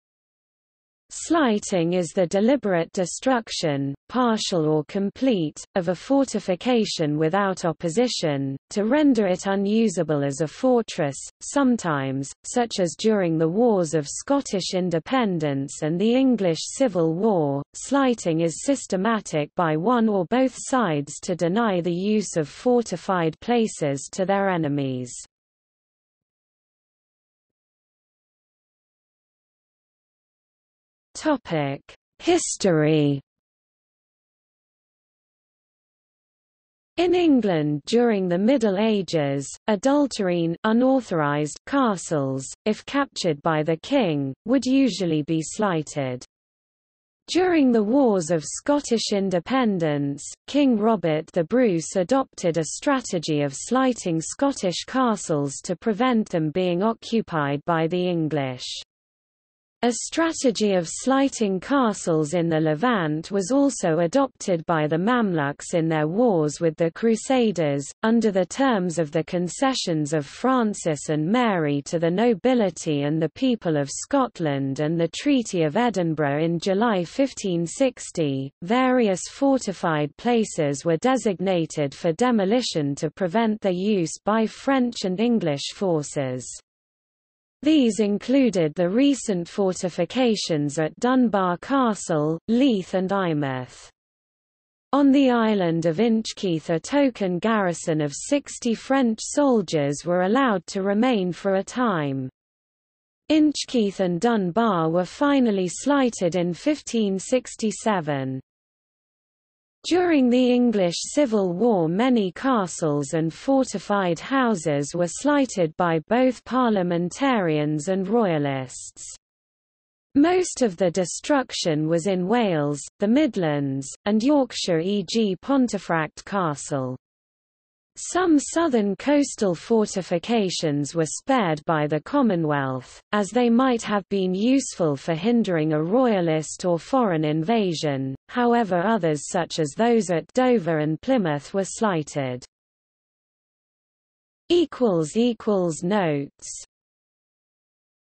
Slighting | Wikipedia audio article
00:00:27 1 History

- Socrates

SUMMARY
Slighting is the deliberate destruction, partial or complete, of a fortification without opposition, to render it unusable as a fortress.Sometimes, such as during the Wars of Scottish Independence and the English Civil War, slighting is systematic by one or both sides to deny the use of fortified places to their enemies.